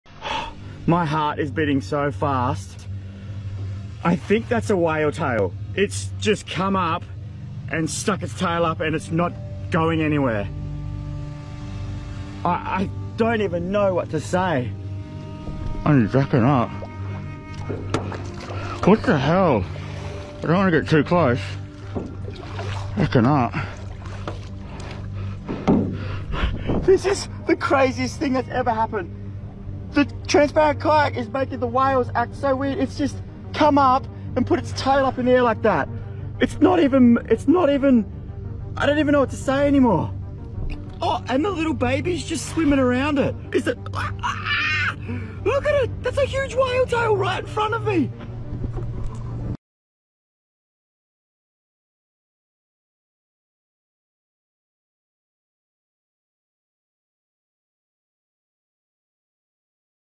Fenomena Langka Paus Jadi Patung dengan Gaya Mirip 'Handstand'
Paus yang berubah menjadi patung atau disebut tail sailing bukan kali ini saja terjadi. Di lepas pantai Brasil, studi paus yang dilakukan pada 1989 dan 2000 terkadang mencatat perilaku tail sailing paus. Setiap peristiwa berlangsung mulai dari beberapa detik hingga yang paling lama 12 menit.⁠
Perilaku ini biasanya dilakukan oleh paus dewasa, induk paus, dan sesekali betina yang sedang hamil. Saat induk paus menunjukkan jadi patung, anak paus biasanya berenang mengelilinya, persis seperti yang direkam oleh Moss. Peneliti menduga, posisi tail sailing membuat induk paus dapat beristirahat sekaligus mengawasi anaknya. Namun, ini masih bersifat hipotesis. Diperlukan penelitian lebih lanjut untuk mengetahui tujuan dari tail sailing.